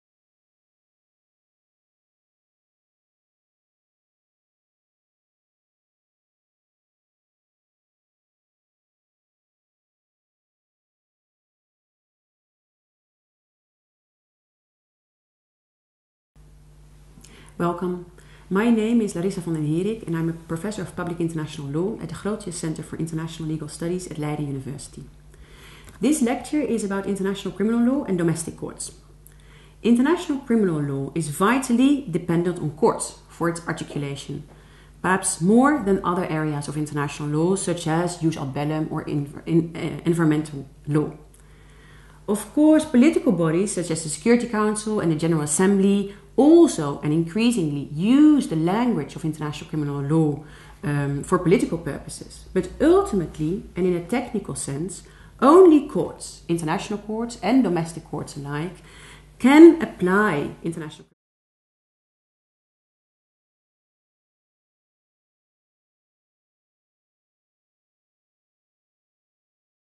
Larissa van den Herik on International Criminal Law and Domestic Courts
Ms. Larissa van den Herik, Professor of Public International Law, Grotius Centre for International Legal Studies, Leiden University